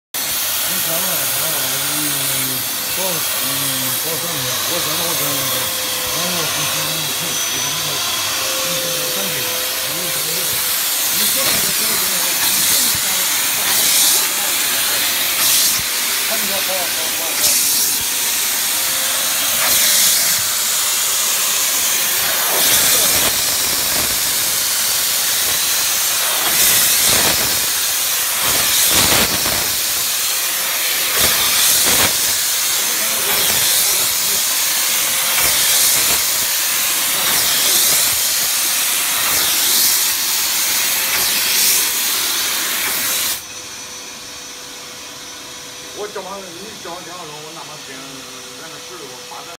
LPG steam car washer + vacuum cleaner clean carpet/sofa/car interior
From this video, you can see this LPG steam car washer is cleaning carpet, by changing a big vacuum wand, it can clean big carpet.
Next video, I will show you how to clean a carpet by a big vacuum wand.